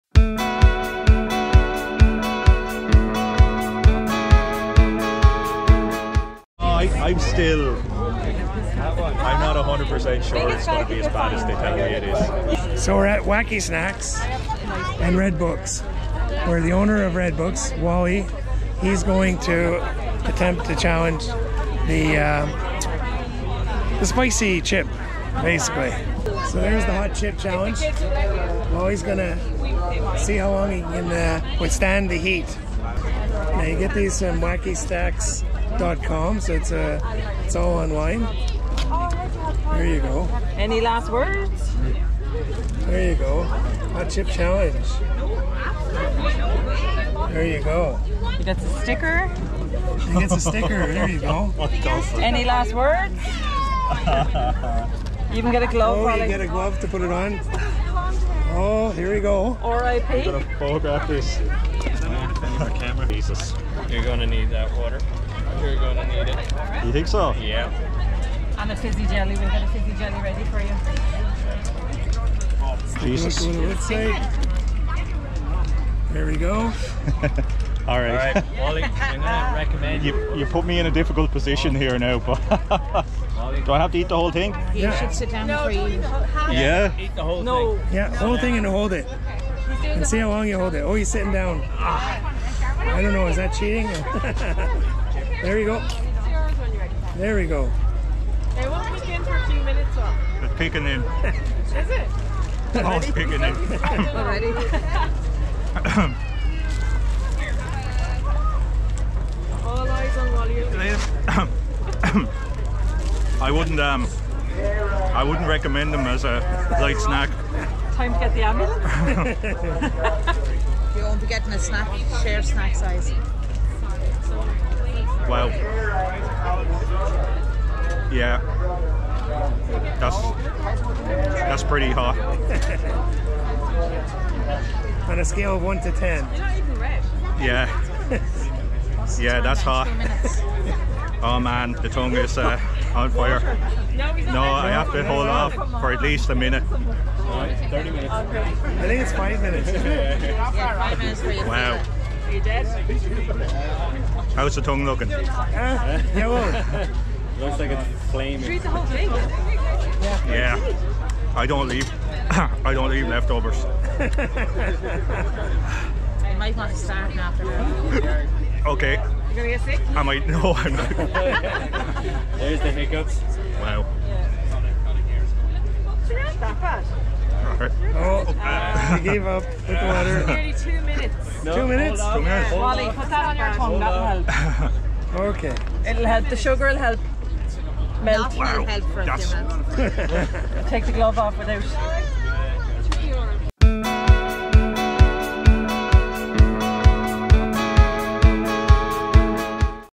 Hot Chip Challange
Wally O Neill from Red Books attempts the Hot Chip Challenge. Carolina Reaper Chip/Crisp

Red Books

Wacky-Snacks.com

At the Bannow Rathangan Agricultural Show
Merchandise